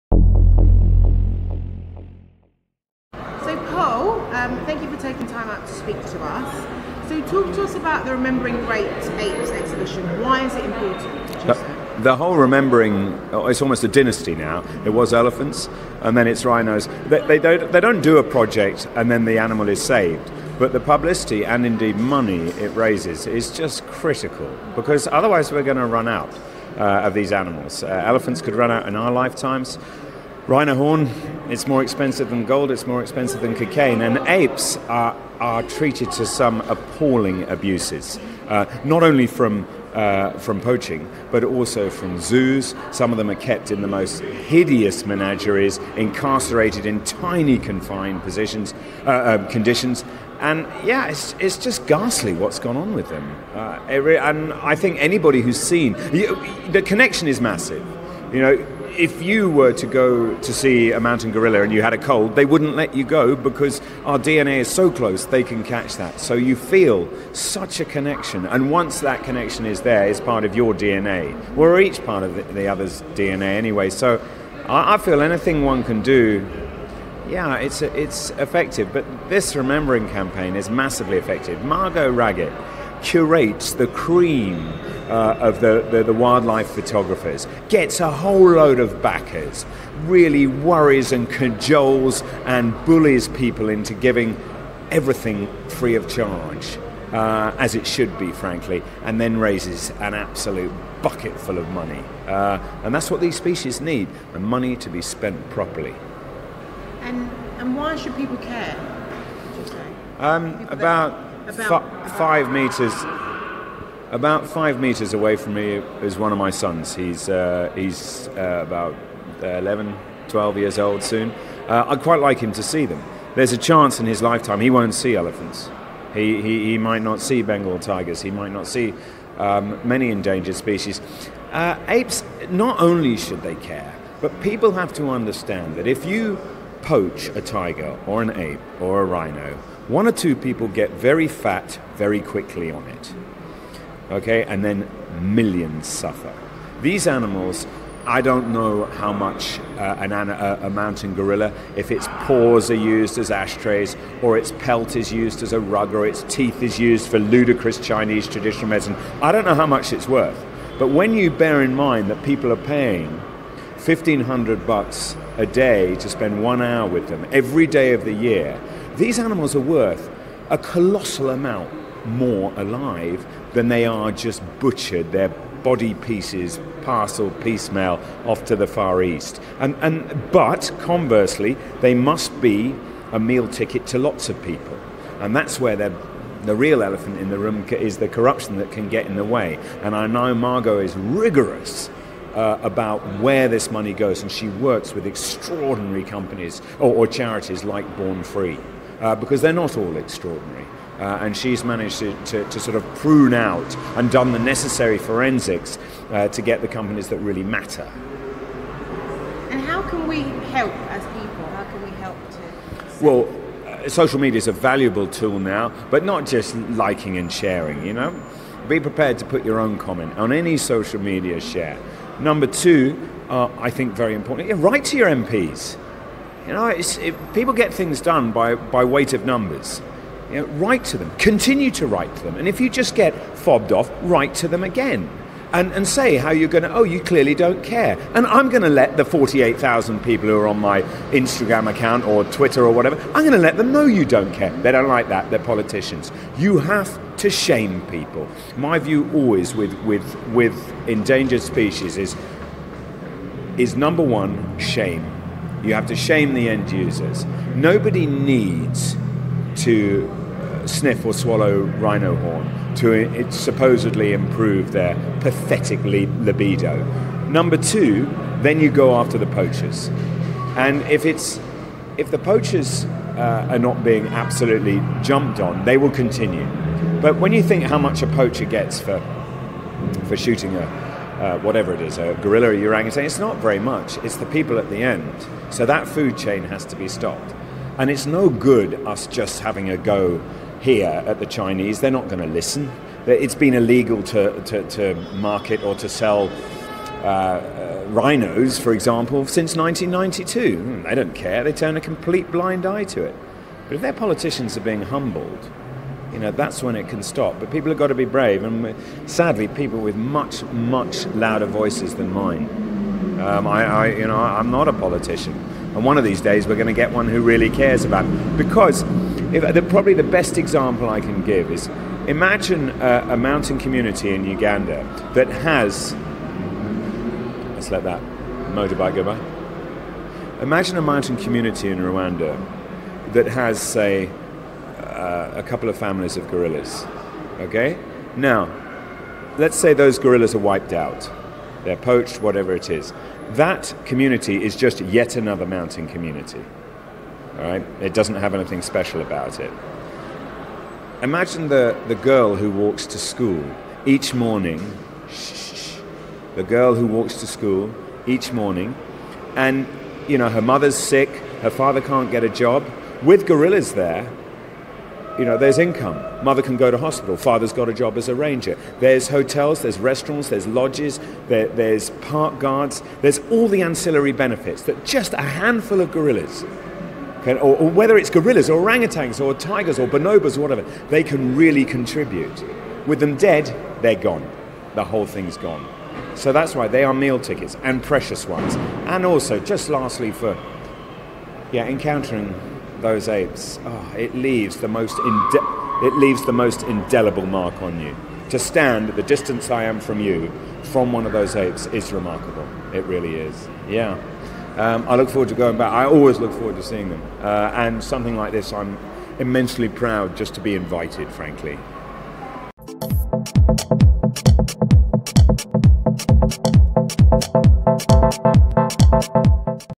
Paul Goldstein | Remembering Great Apes
Remembering Great Apes will be the third book in the highly acclaimed Remembering Wildlife series of charity books and has been edited by Ian Redmond OBE and features a foreword by Jane Goodall, PHD, DBE. The book will be full of beautiful images donated by top wildlife photographers such as Tim Laman, Anup Shah, Art Wolfe, Jonathan & Angela Scott and Michael Poliza, with an aim to raise awareness of the plight facing gorillas, chimpanzees, bonobos and orangutans and also to raise funds to protect them.